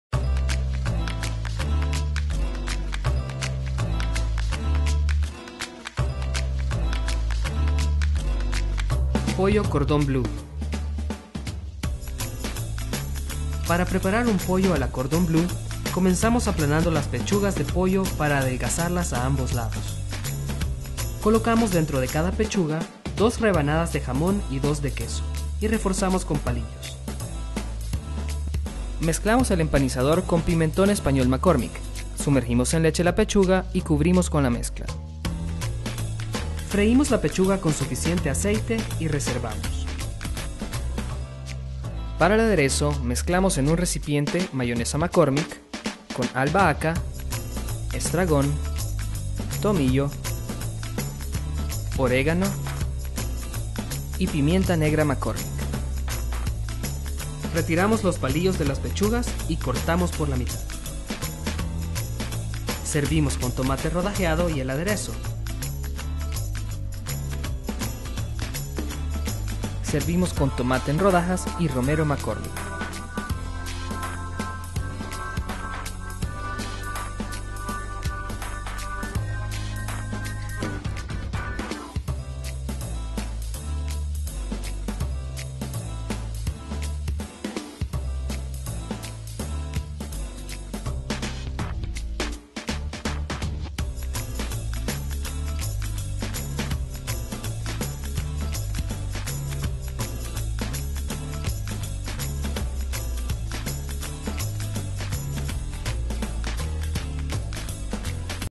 Pechuga a la Cordon Blue
Aprende a preparar de una manera muy sencilla una pechuga a la Cordon Blue y acompaña con aderezo a base de Mayonesa McCormick y el delicioso toque de las hierbas McCormick.